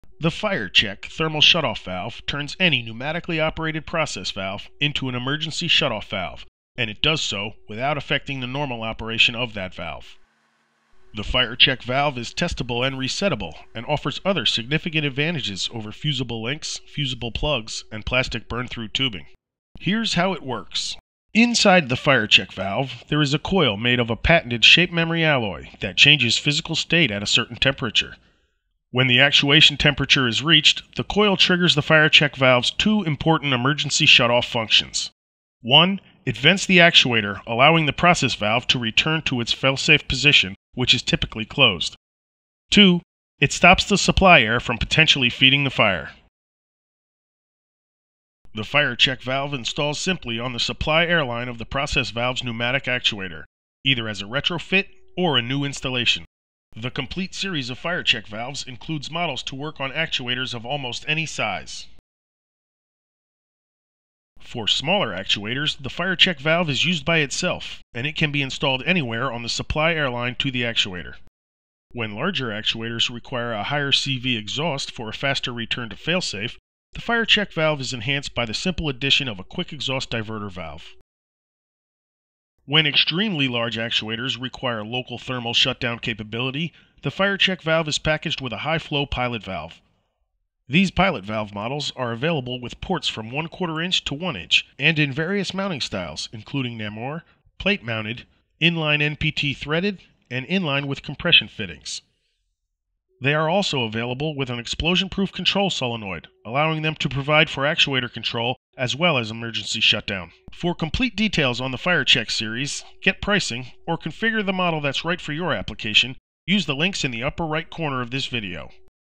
FireChek Series Overview
A brief overview of the entire series of FireChek Thermal Shut-off valves including the new quick exhaust models and pilot valve models for use on large actuators.